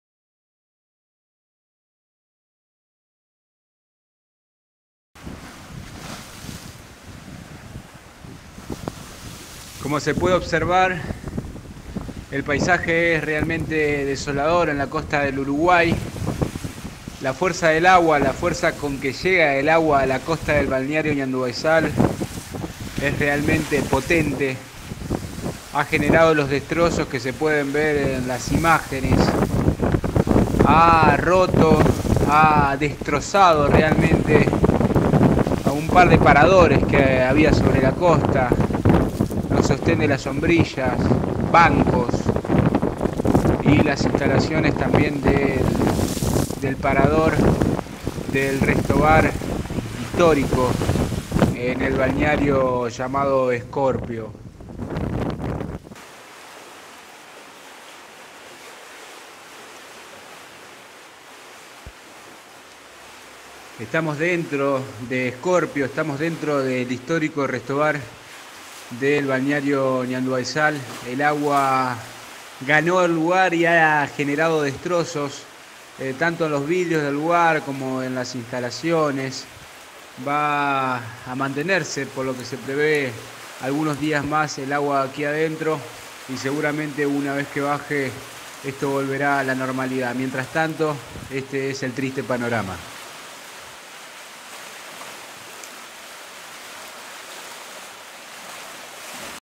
Destrozos en el Balneario Ñandubaysal
La crecida del río Uruguay y los fuertes vientos que azotan la costa, generaron destrozos en el lugar.